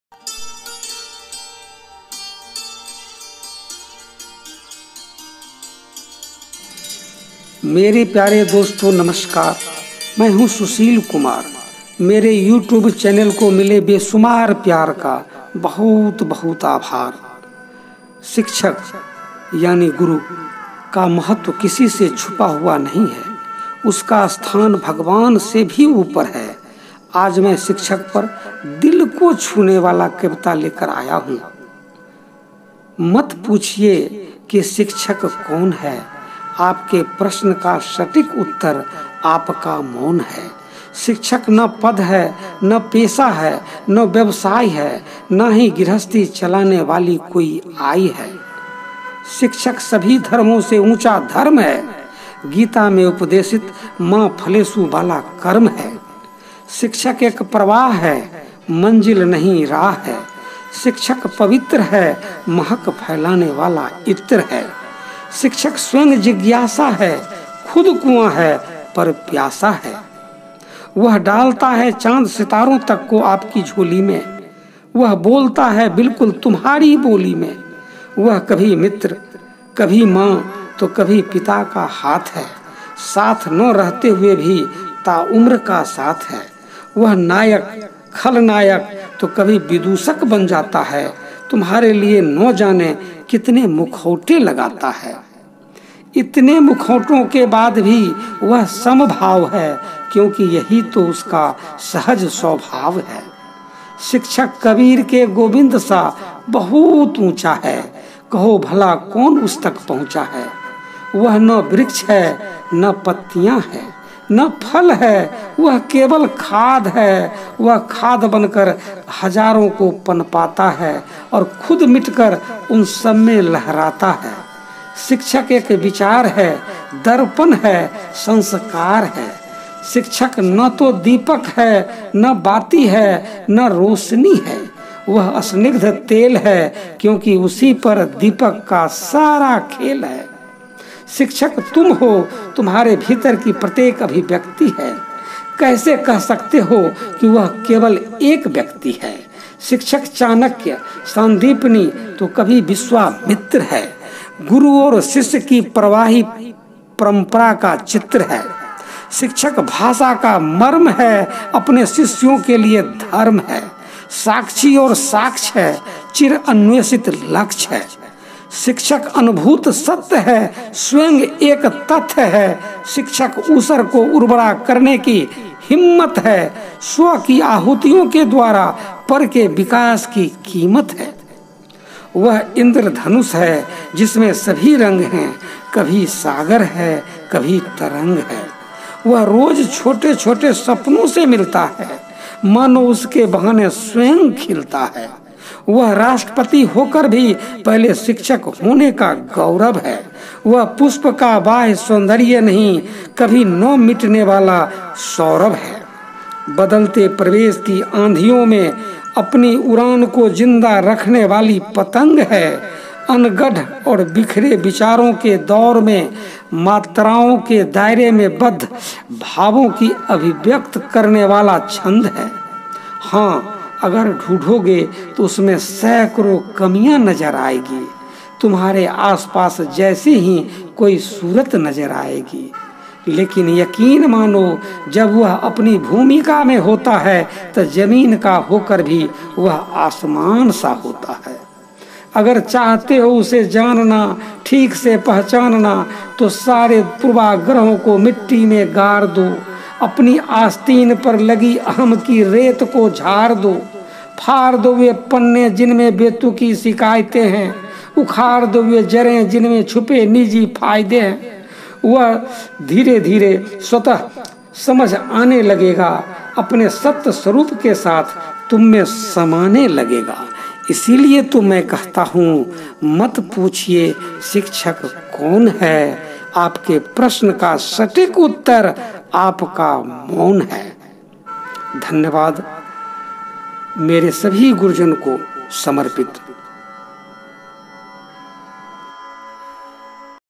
दिल को छू लेने वाली कविता ' शिक्षक' - सुशील कुमार सिंह
सभी शिक्षकों को समर्पित एक दिल को छू लेने वाली कविता 'शिक्षक कौन'- लेखक एवं आवाज़- सुशील कुमार सिंह